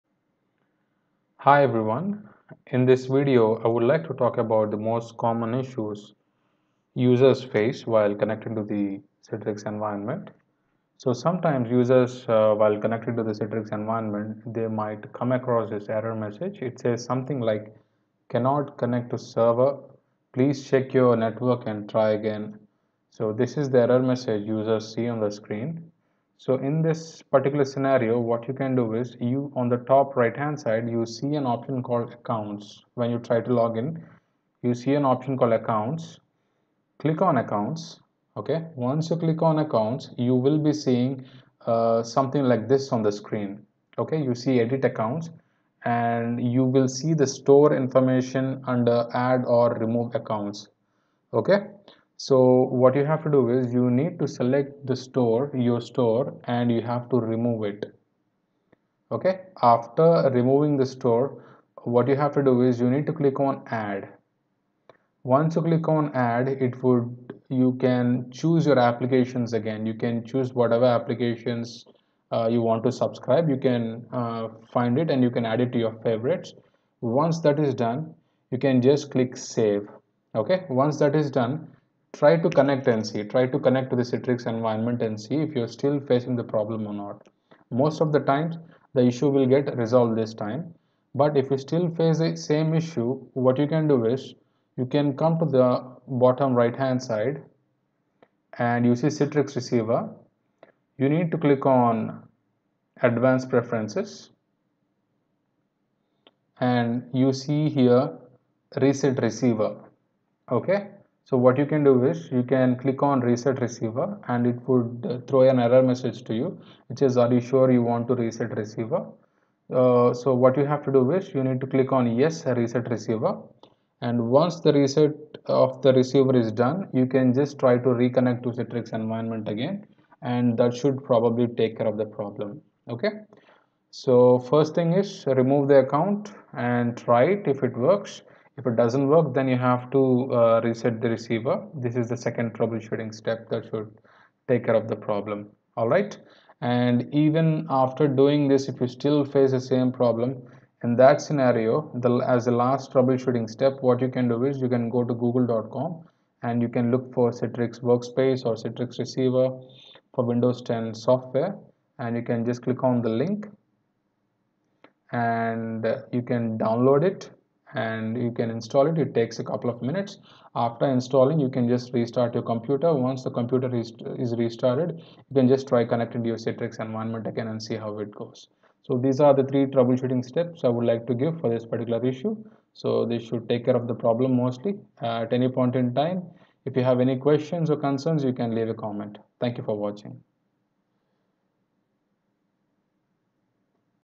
Cannot connect to server Citrix- Please check your network and try again | Citrix Receiver/Workspace
In this video, I showed how to solve the problem when you see an error 'Cannot connect to server Citrix- Please check your network and try again'

Open Citrix Workspace (or Citrix Receiver).
Select Menu on the right hand side of the Citrix Workspace (or Receiver) window, then choose Accounts.

Select your Primary account (there should be only one),] and click the Remove... button below to remove it.
Click OK to confirm deletion of the account. You will automatically be logged out of AppsToGo.
Return to Citrix Window and select Menu, and then Accounts.
Click the Add... button to add an account.

Enter your details and,
Click Add.

Enter your Internet ID and Password then click Log On. You are now logged in.

Choose OK to close the window.

Thanks!